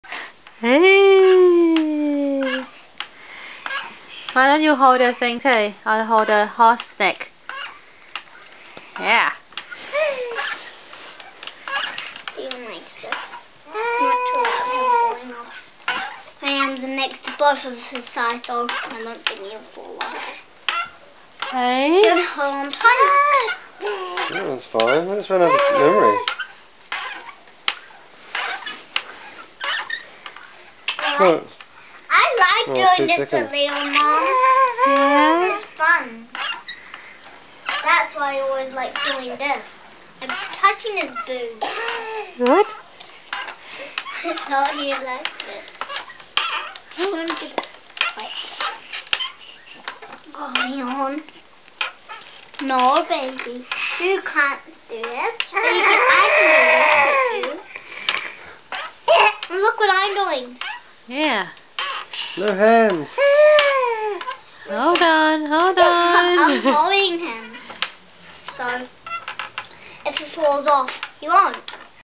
Rocking horse
Delfina and Leon ride the rocking horse